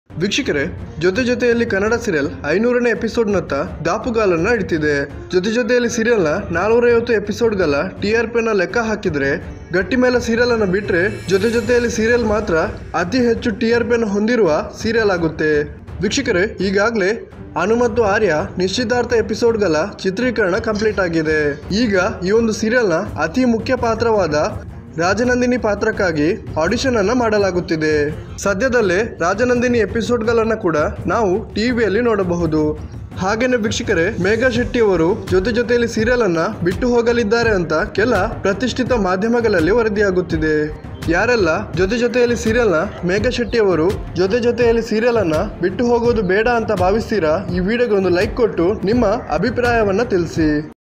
ನಡಿಯುತ್ತಿದೆ ರಾಜನಂದಿನಿಯ ಹುಡುಕಾಟ | Jothe Jotheyali kannada serial Completes 500 Episode Shoot
Jothe Jotheyali Kannada serial is the most popular serial of zee kannada this serial has huge starring actress like megha shetty aniruddh sir and aryavardhan etc. Megha shetty is going to play role of rajanandini story its not confirmed yet. Jothejotheyali serial completes 500 episode shoot today.